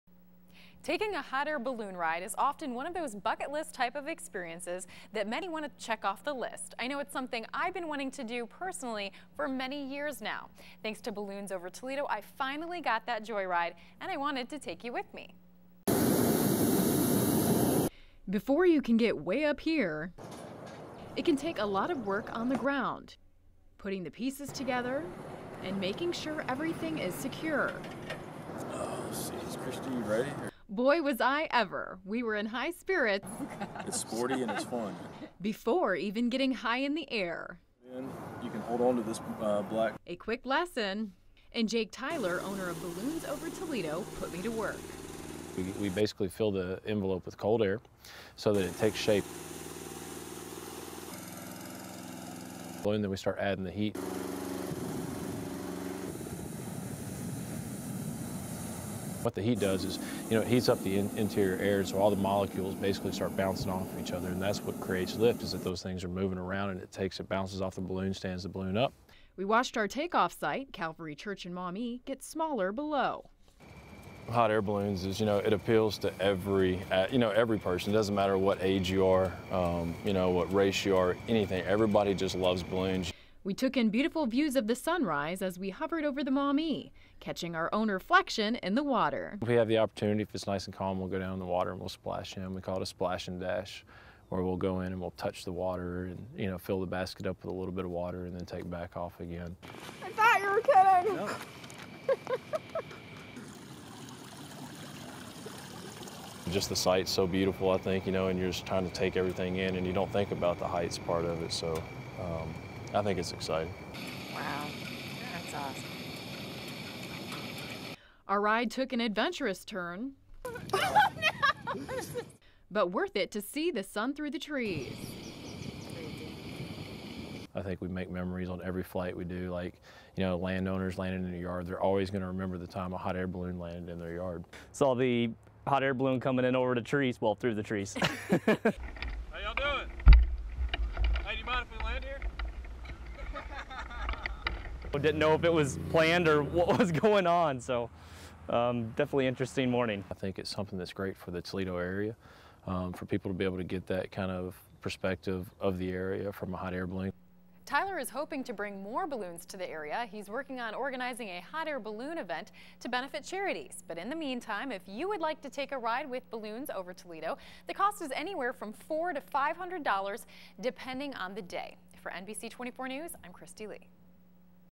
A Trip "UP" to See Our City
Jake Tyler wants to bring ballooning back to the Toledo Area. He says there used to be ballooning festivals in town, and now it's rare to see one overhead.
He says ballooning puts a smile on everyone's face-whether in the basket or spotting it from below. He plans to bring back ballooning and use it to benefit area charities.
Our crew brought the unique perspective to our viewers and took them along for the ride.